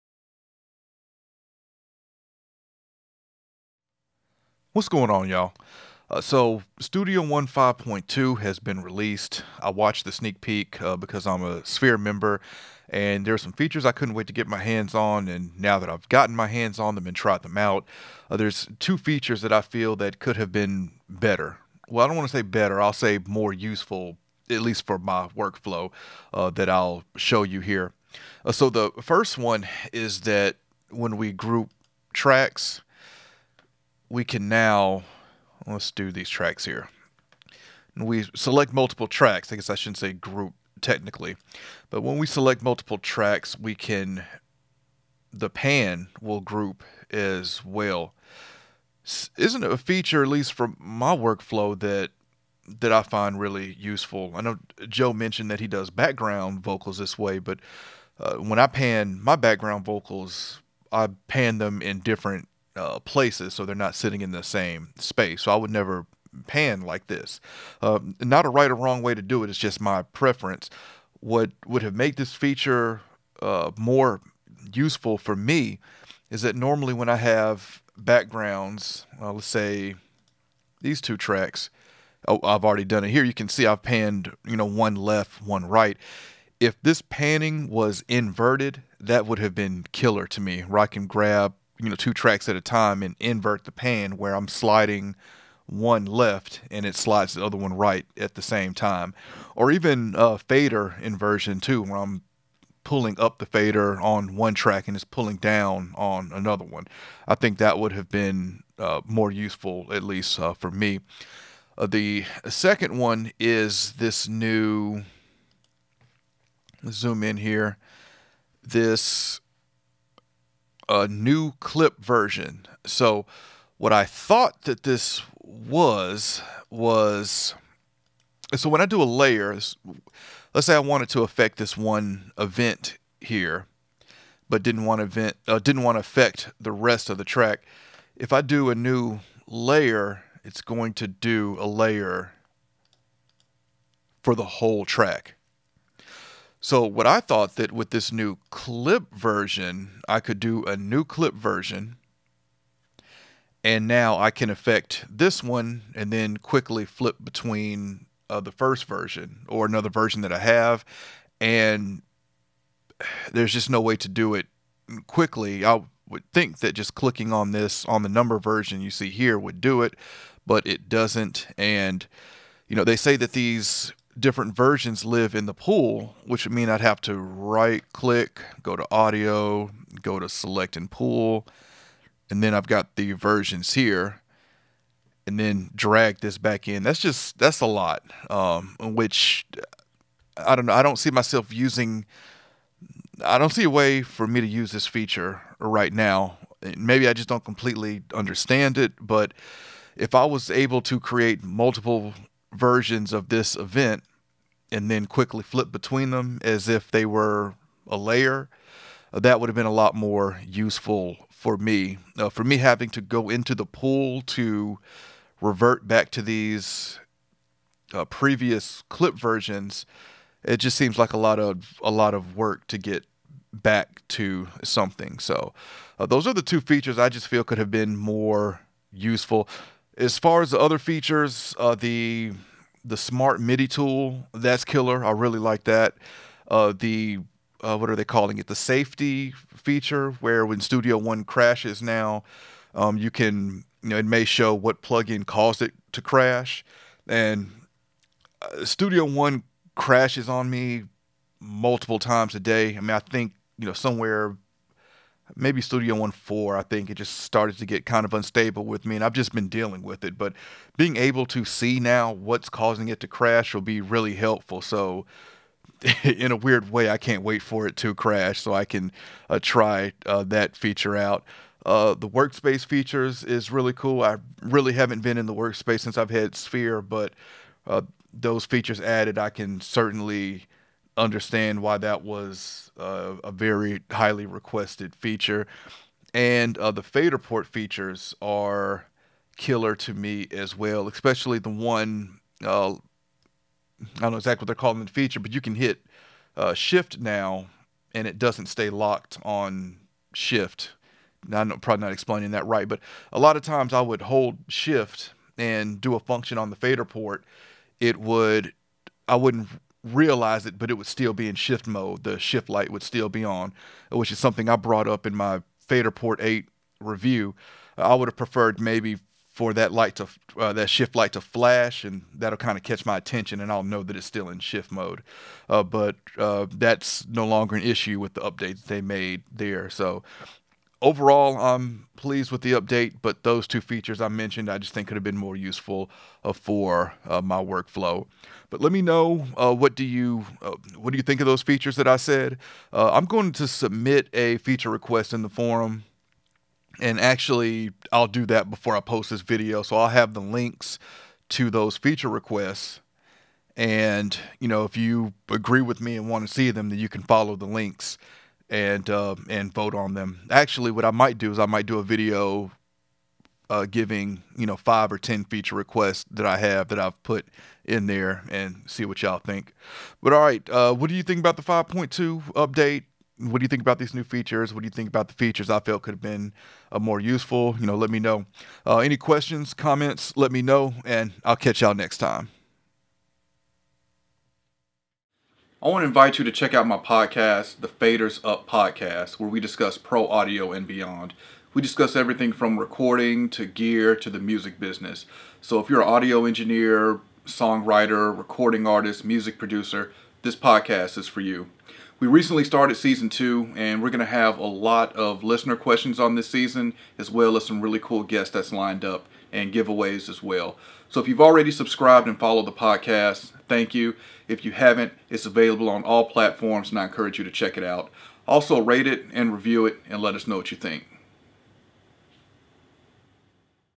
PreSonus Studio One 5.2 - 2 new features that could have been better
In this video I show two of the new features added in studio one 5.2 I felt that could have been better. If you agree with me here are the links to vote on the feature requests I submitted.

Also check out the Faders Up Podcast